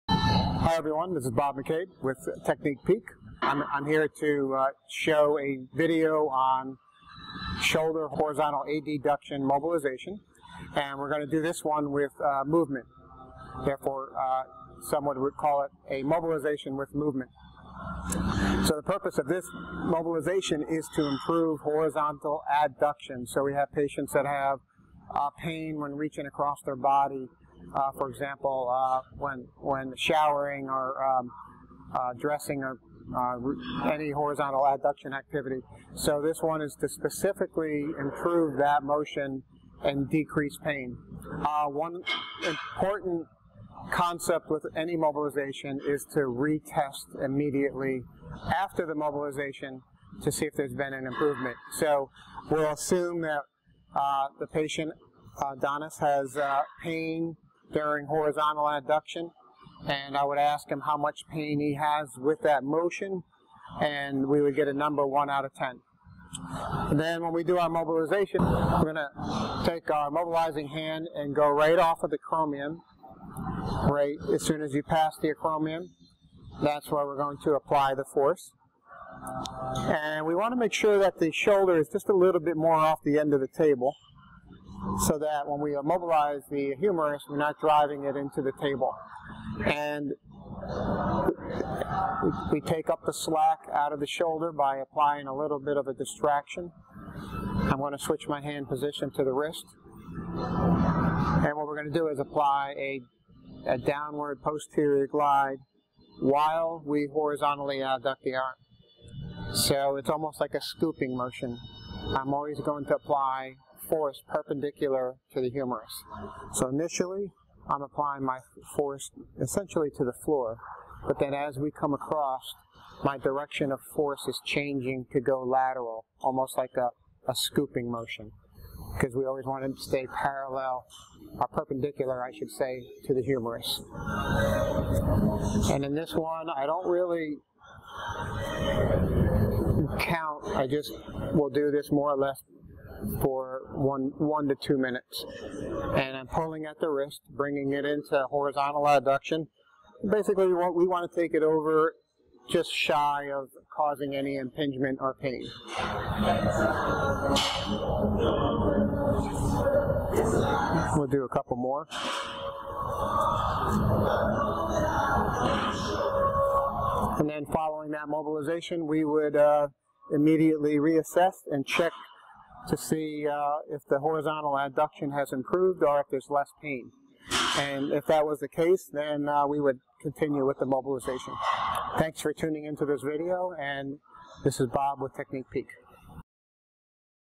Shoulder Horizontal Adduction Mobilization with Movement | Technique Peek Series
This Technique Peek Series Video features Robert McCabe, MS, DPT, OCS demonstrating how to perform a mobilization with movement technique to the shoulder in order to improve horizontal adduction and decrease pain with movement.